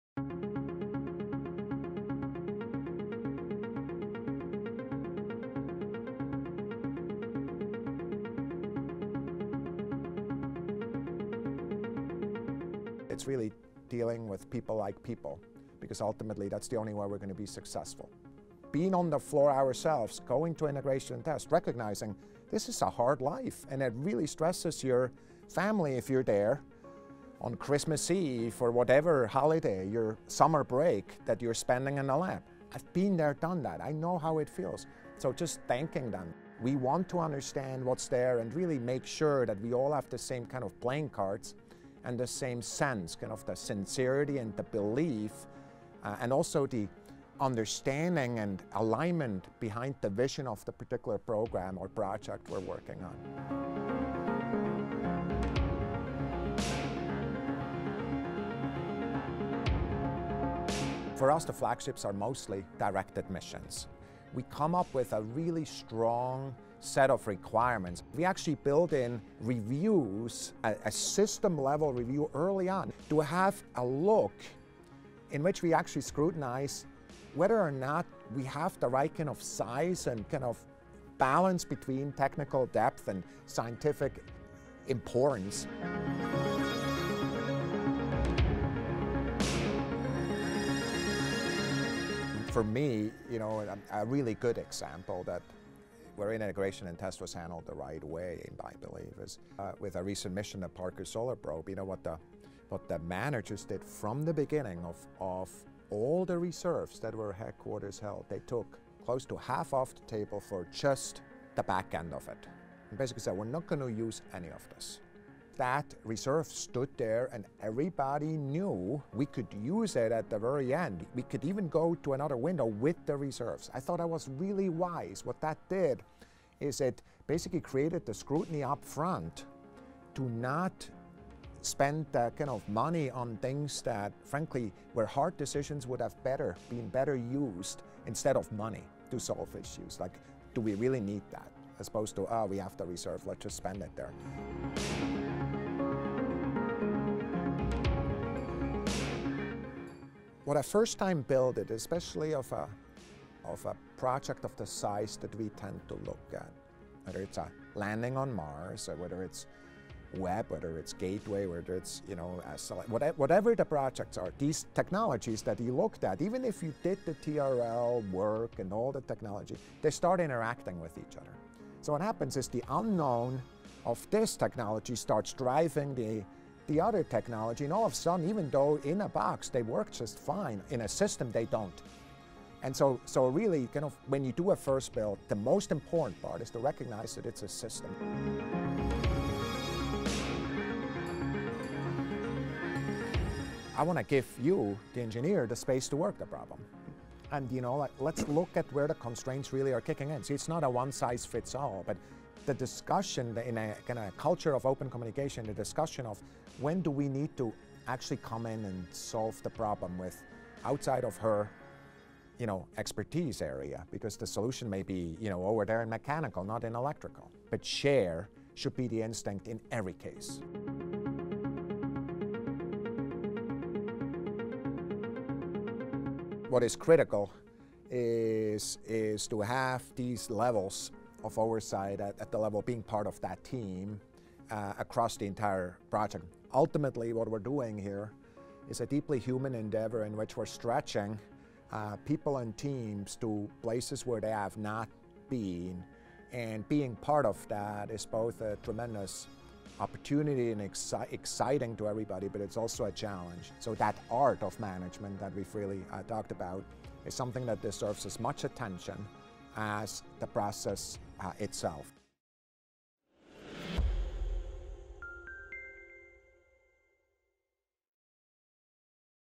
Thomas Zurbuchen on Lessons Learned from Large NASA Projects
Thomas Zurbuchen, associate administrator for NASA’s Science Mission Directorate, is responsible for directing and overseeing the nation’s space research program in Earth and space science.

appel.nasa.gov